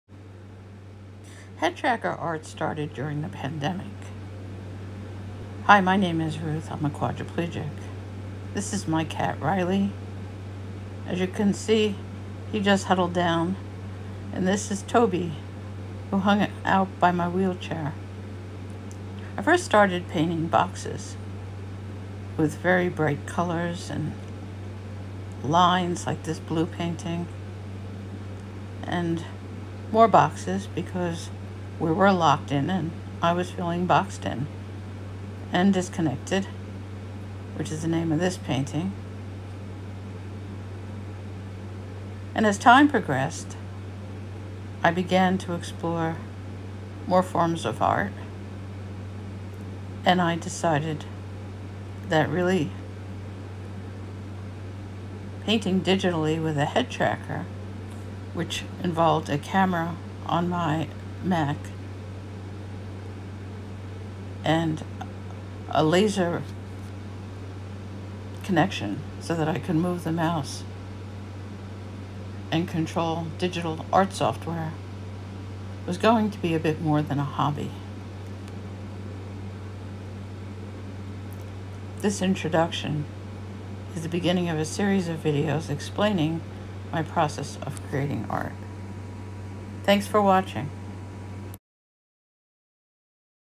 Headtracker Art part one￼: The beginning￼
This video talks about my early Artwork as well as how I started to create art with a headtracker. It explains how the headtracker works and when I realized that painting was more than a hobby

To support my art and see what I’m currently working on please visit